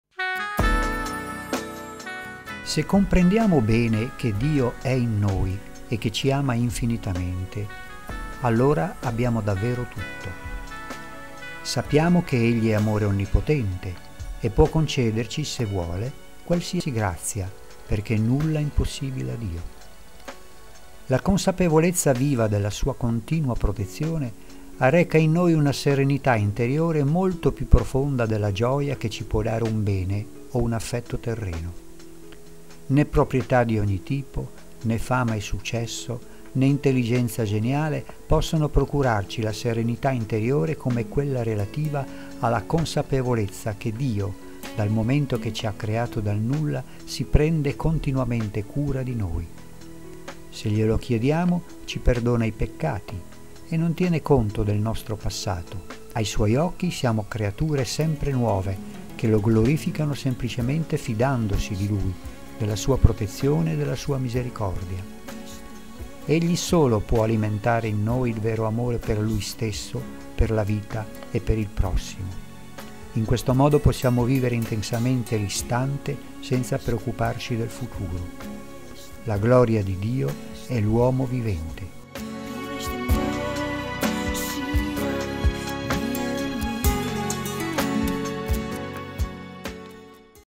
QUANDO SI HA DIO SI HA TUTTO
Se comprendiamo bene che Dio è in noi e che ci ama infinitamente allora abbiamo davvero tutto.
Sappiamo che Egli è amore Onnipotente e può concederci, se vuole, qualsiasi grazia perché “Nulla è impossibile a Dio”.
La consapevolezza viva della sua continua protezione arreca in noi una serenità interiore molto più profonda della gioia che ci può dare un bene o un affetto terreno.
Né proprietà di ogni tipo, né fama e successo, né intelligenza geniale possono procurarci la serenità interiore come quella relativa alla consapevolezza che Dio, dal momento che ci ha creato dal nulla, si prende continuamente cura di noi. Se glielo chiediamo, ci perdona i peccati e non tiene conto del nostro passato: ai suoi occhi siamo  creature sempre nuove che lo glorificano semplicemente fidandosi di Lui, della sua protezione e della sua Misericordia. Egli solo può alimentare in noi il vero amore per Lui stesso, per la vita e per il prossimo. In questo modo possiamo vivere intensamente l’istante senza preoccuparci del futuro.
“La gloria di Dio è l’uomo vivente”

CONCETTA BERTOLI - La donna che vide la terza guerra mondiale

GESÙ CHIEDE TOTALE FIDUCIA IN LUI (nel "Colloquio interiore" di suor Maria della Trinità)